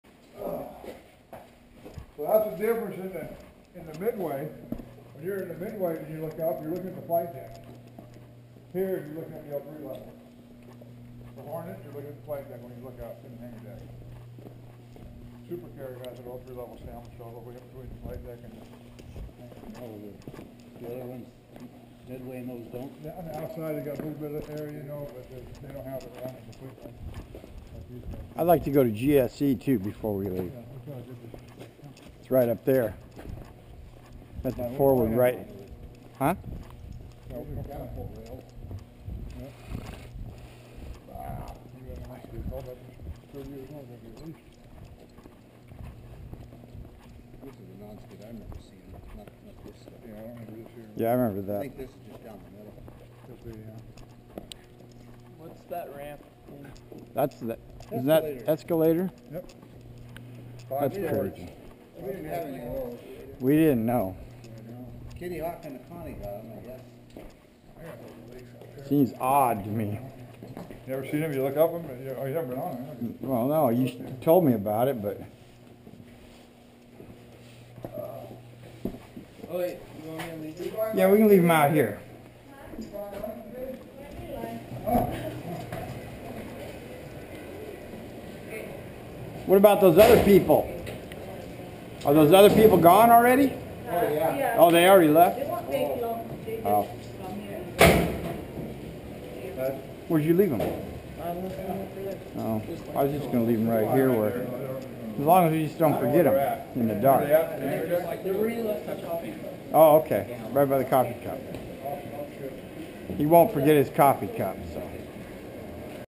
USS RANGER raw video part 5
This is all the raw video from when we filmed the final tour on RANGER. Enjoy it is  7 to 8 hours worth of raw video.